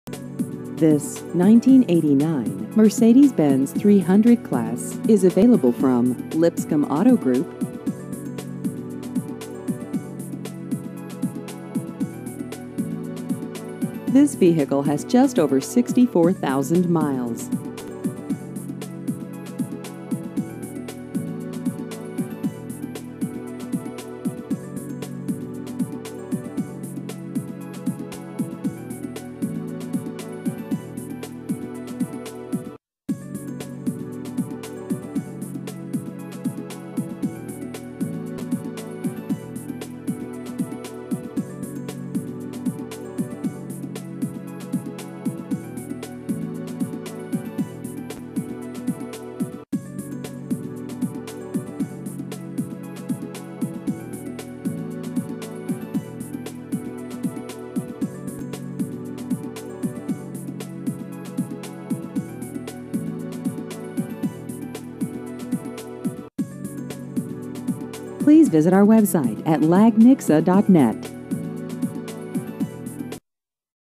1989 Mercedes-Benz 300-Class Used Cars Nixa MO
This 1989 Mercedes-Benz 300-Class is available from Lipscomb Auto Group.